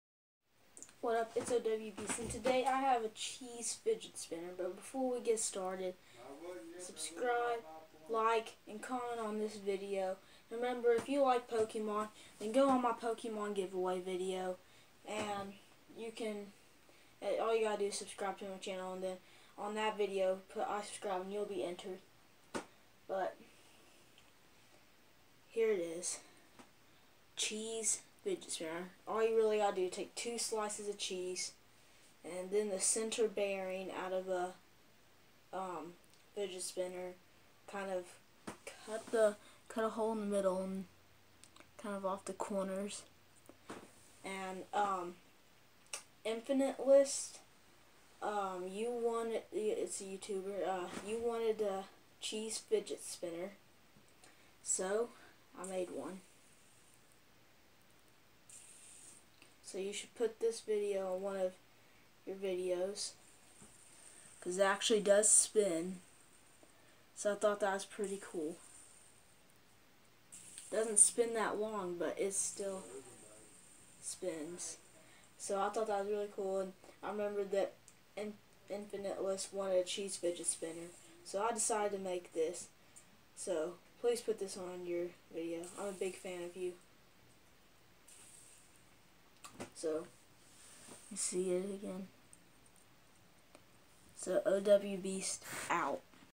cheese fidget spinner
OMG! I made a cheese fidget spinner